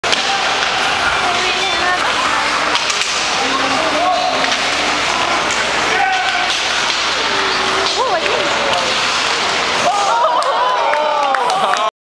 Kristoff Kontos nice goal at PAK charity game
Dave Tataryn's Charity Hockey game in Midland, Ontario raises money for Kids. PAK (Players Assisting Kids) raised money for 4 local charities. Team White won the game 9-8 in a shootout after regulation play.